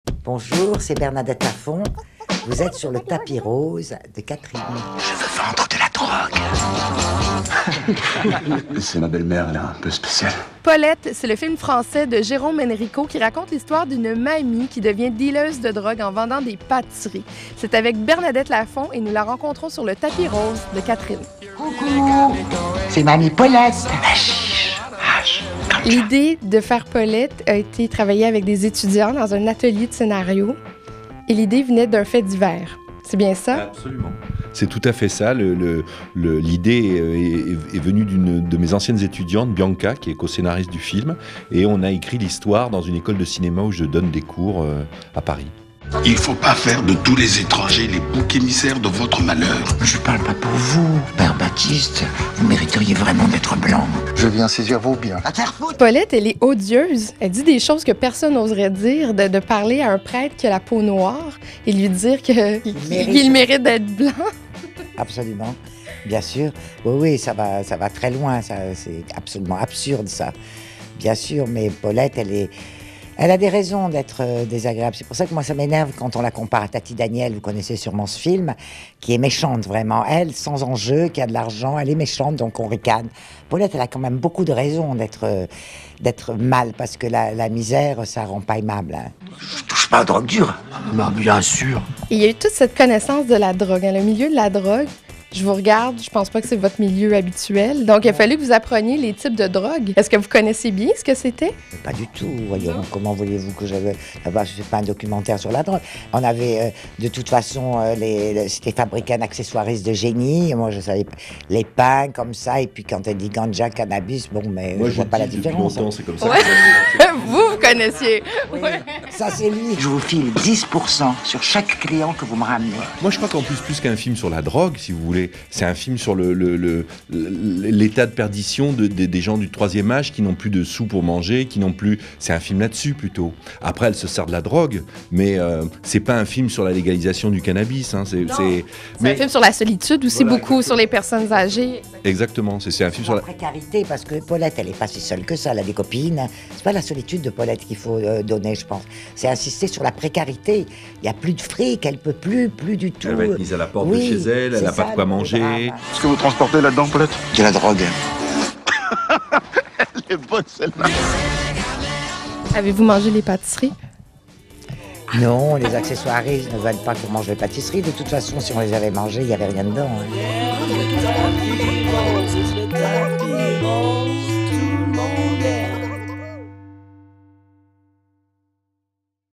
Bernadette Lafont: une mamie trafiquante de drogue
Paulette c'est une Mamie qui, pour subvenir à ses fins de mois, décide de dealer de la drogue dans son quartier. Ancienne boulangère, elle intégrera le cannabis à ses meilleures pâtisseries. Le Tapis rose rencontre l'interprète de Paulette, la comédienne Bernadette Lafont à Paris !